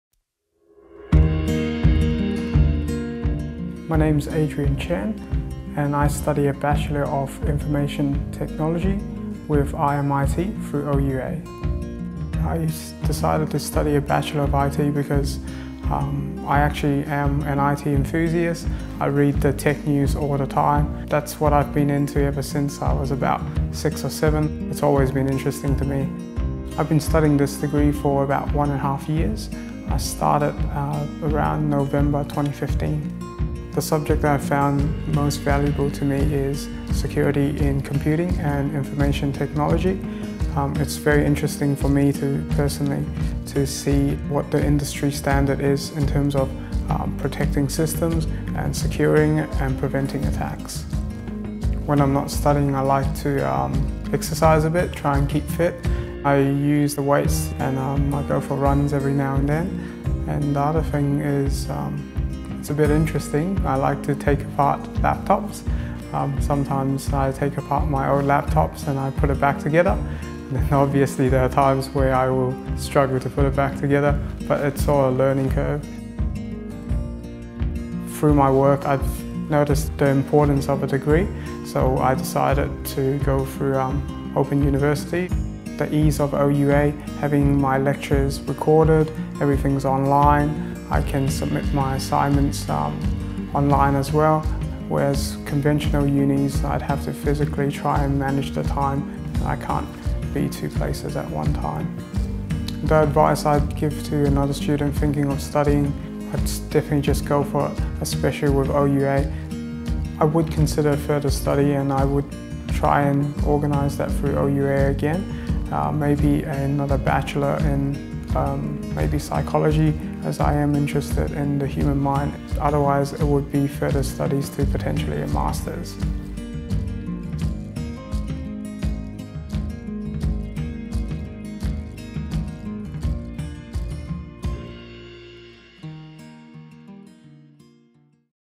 Adrian's story - Bachelor of Information Technology
From as early as he can remember, Adrian has taken a keen interest in Technology. His ultimate career goal is to work with artificial intelligence, and everything he’s exploring, whether at uni or on his own time, gets him closer to this dream. It’s no surprise that Adrian was drawn to a broad and varied degree like RMIT’s Bachelor of Information Technology. The program exposes him to a number of fascinating areas, from system security and attack prevention, to web design and user experience. With the freedom to study on his own terms, he’s in full control and loving every day of it.

Connect with Open Universities Australia